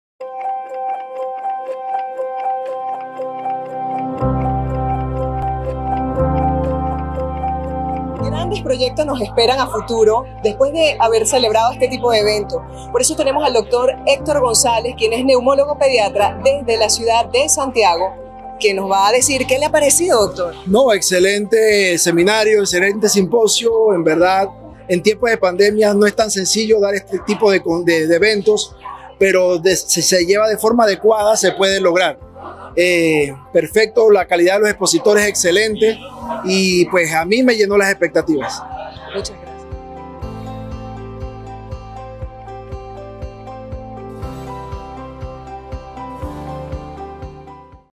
Entrevista Dr. Héctor Gonzalez
Impresiones del Especialista en Neumología Pediátrica sobre el 3 simposio de neumología - Neumoclinic 2021